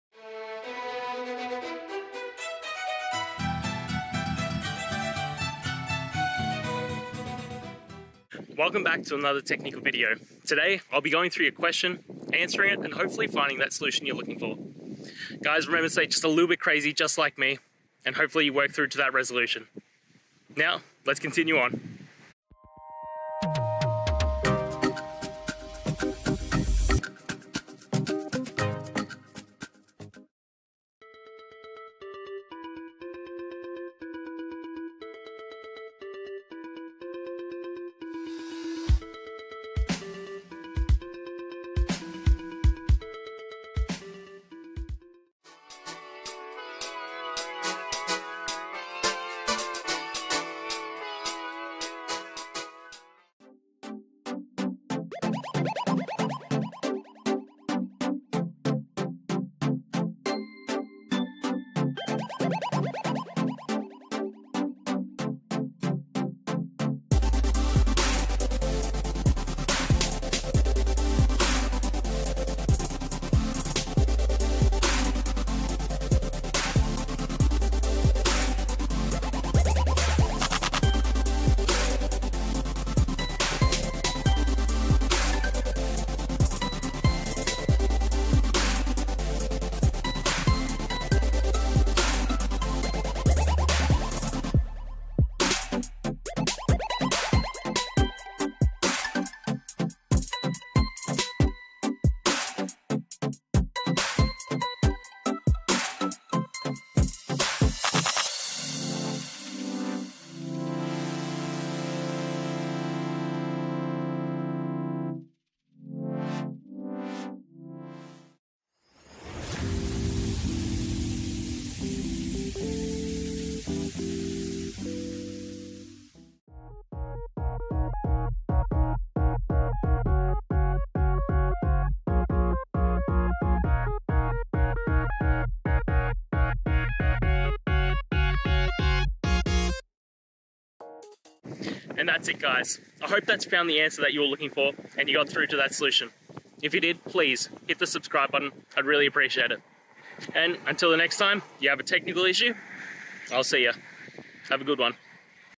Plot histogram using two columns (values, counts) in python dataframe
python: Plot histogram using two columns (values, counts) in python dataframe

Related to: python, pandas, matplotlib, plot